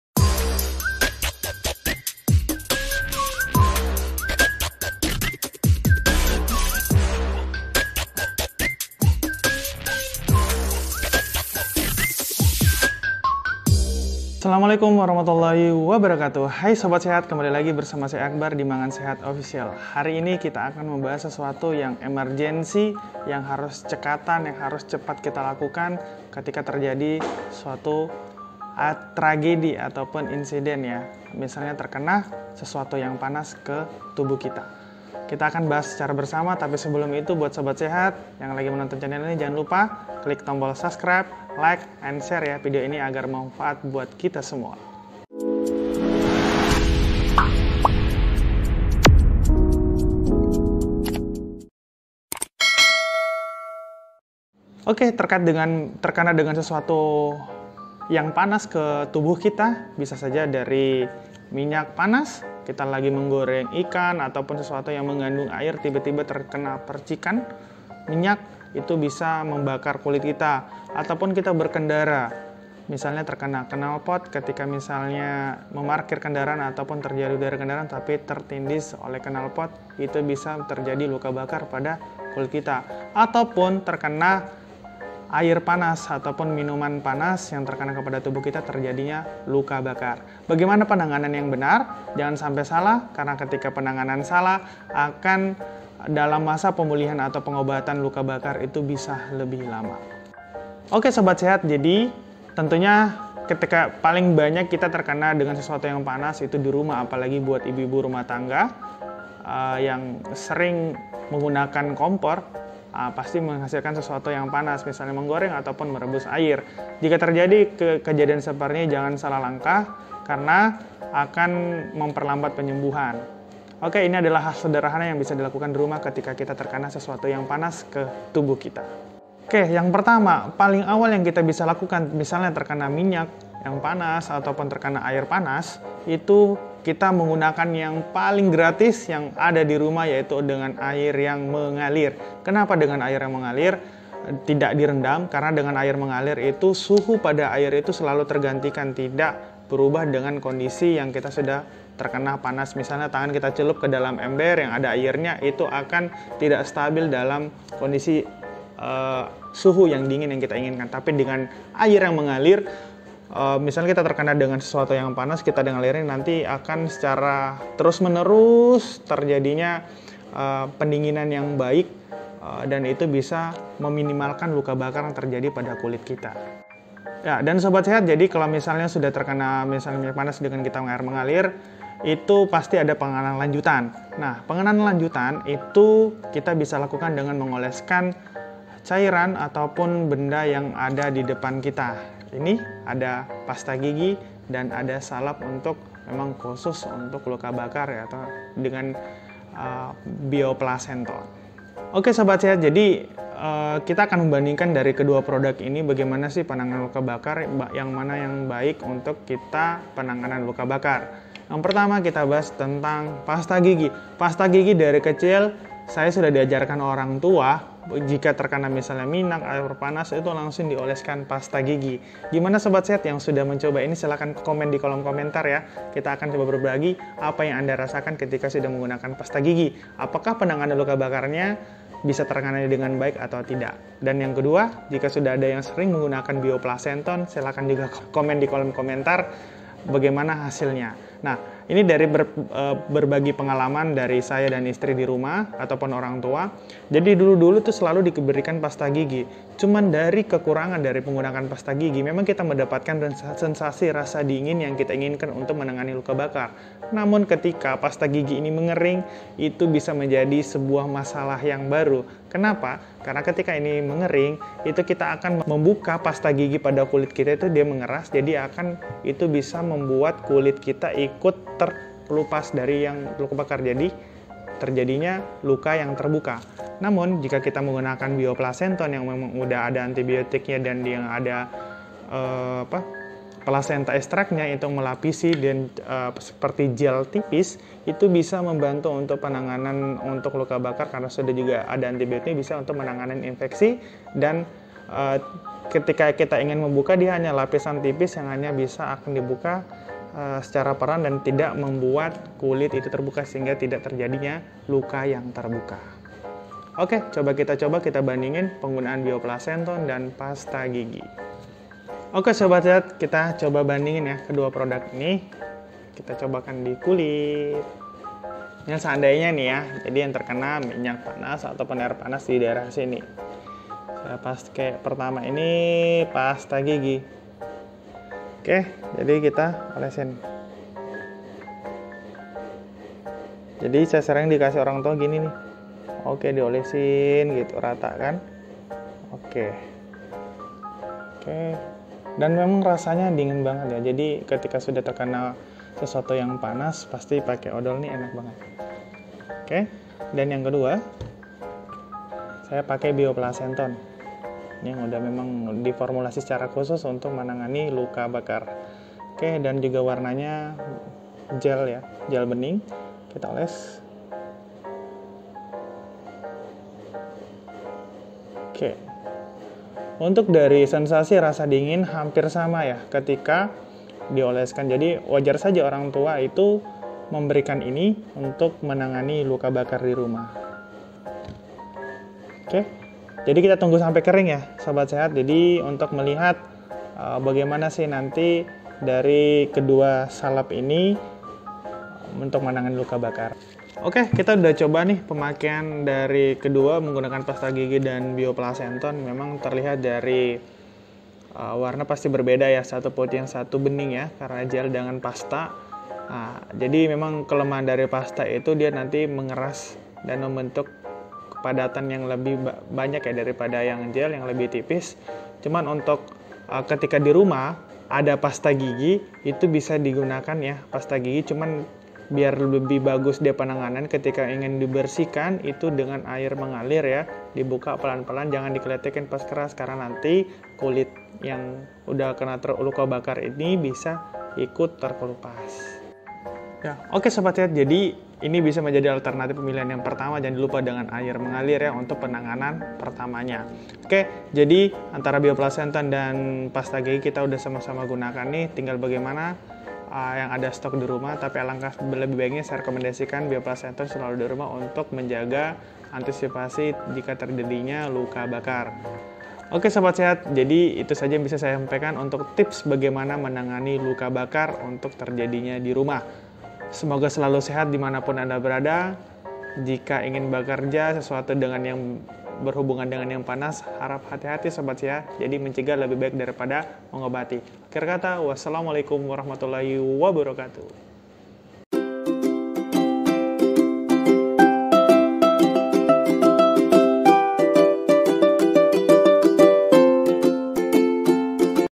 Tips Penanganan Luka Bakar di Rumah | Wajib Tahu !!!
Cedera akibat luka bakar dapat terjadi di mana saja, termasuk di rumah. Jika bukan Anda yang mengalaminya, bisa jadi Anda merupakan orang terdekat yang dapat membantu korban. Maka dari itu, penting untuk mengetahui cara memberikan pertolongan pertama pada luka bakar.